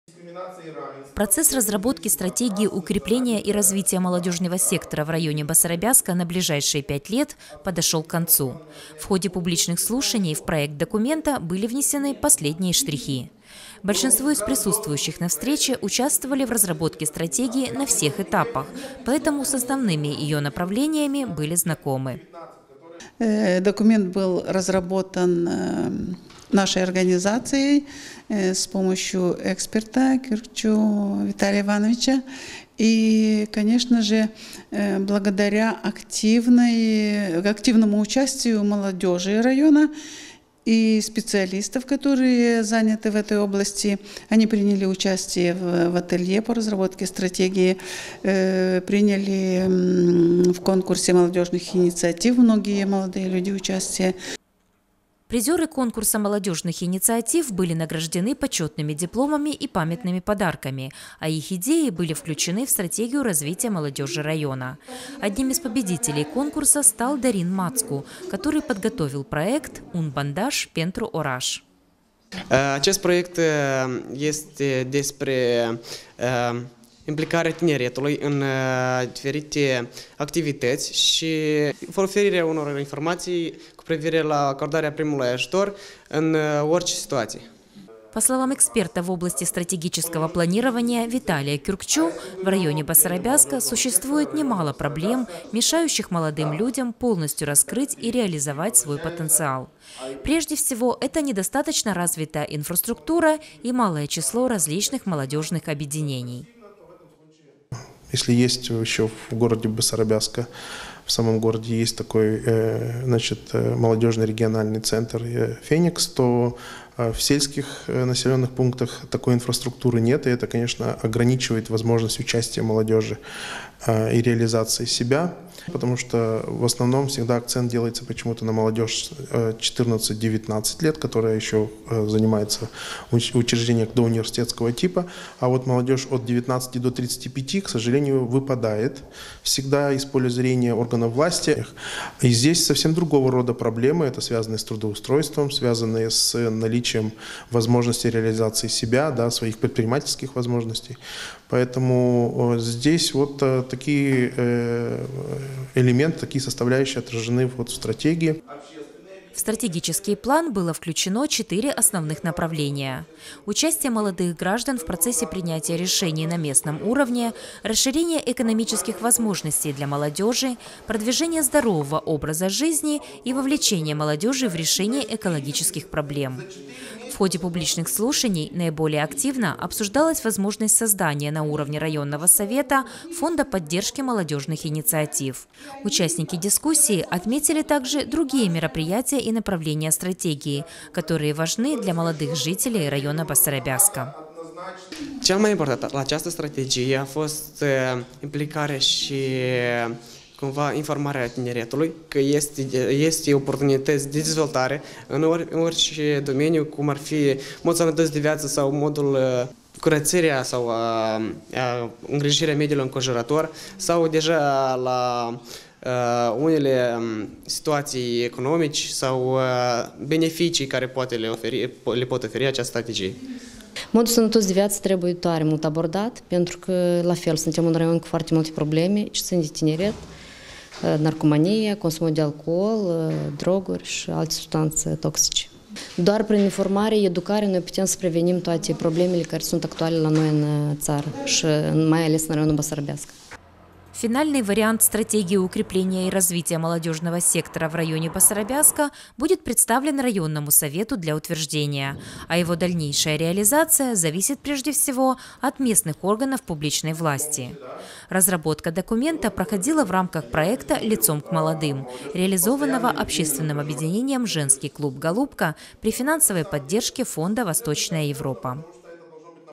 В районе Басарабяска появилась «Стратегия развития молодежи»: от кого зависит её реализация?
Процесс разработки Стратегии укрепления и развития молодежного сектора в районе Басарабяска на ближайшие 5 лет подошел к концу. В ходе публичных слушаний в проект документа были внесены последние штрихи.